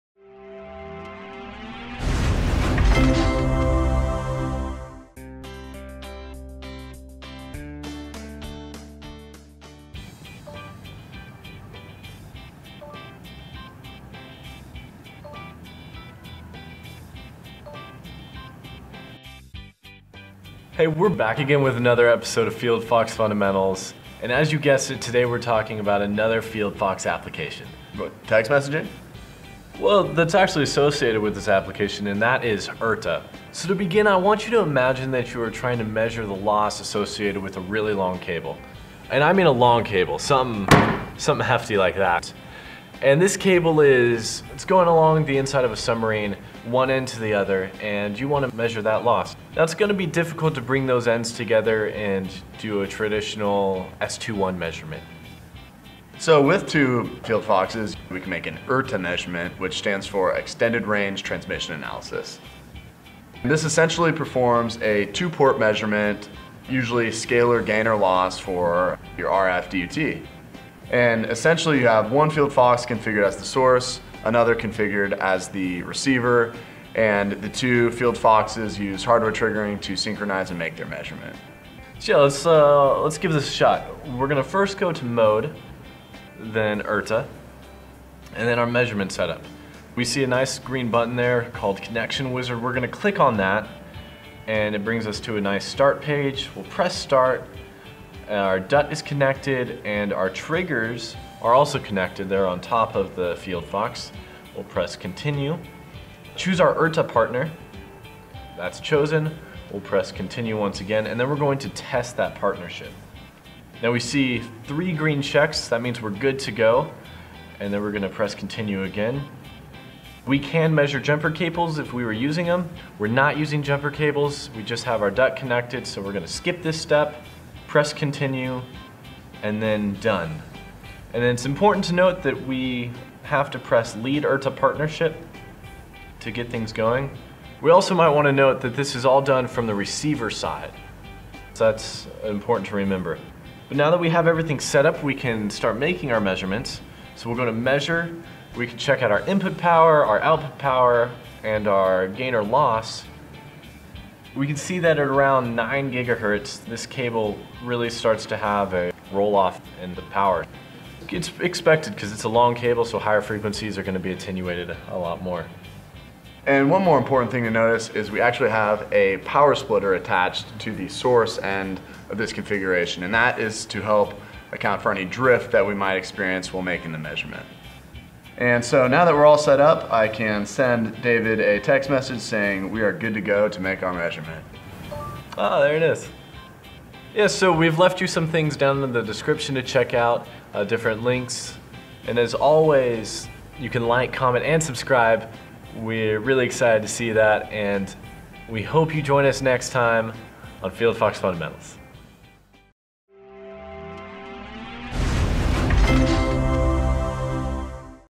FieldFox Fundamentals, Episode 9: Extended Range Transmission Analysis (ERTA)
Extended Range Transmission Analysis or ERTA, is a FieldFox application that lets you measure cables that are either hard to connect at both ends, or are too long where it doesn’t make sense for them to be connected to the same instrument.